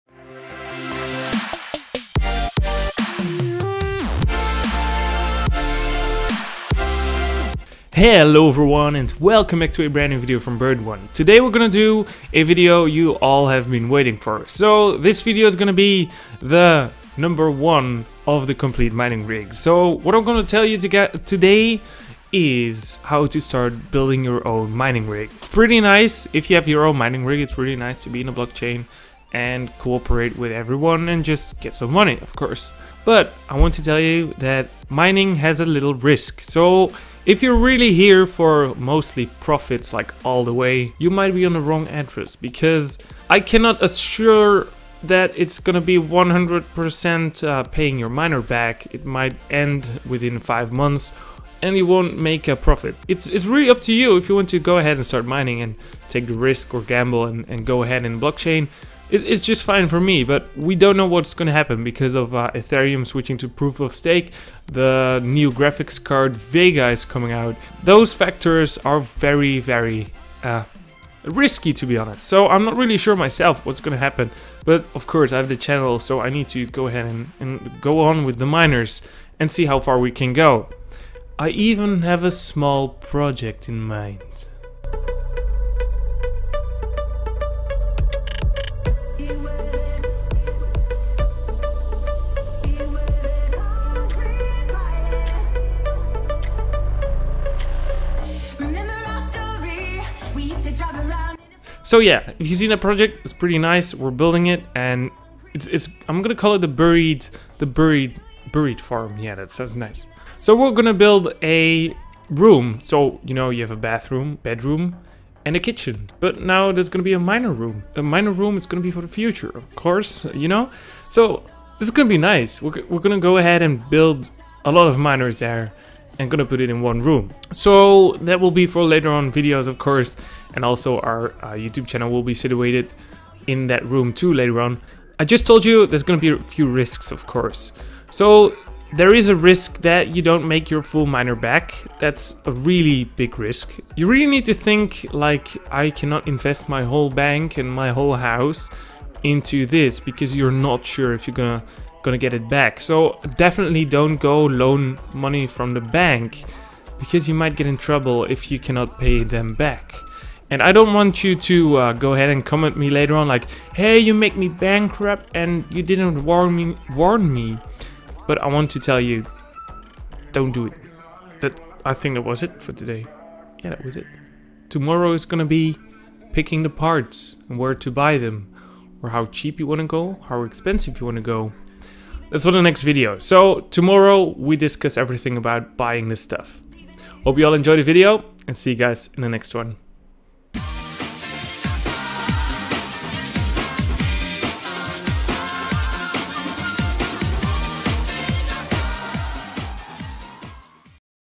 1# Risky it is. - The Complete Mining Rig Build Guide
The first step is just a quick reminder for you to start thinking about starting to invest into mining, its getting pretty risky because we dont know what the future holds with Ethereum and the new Vega GPU comming out...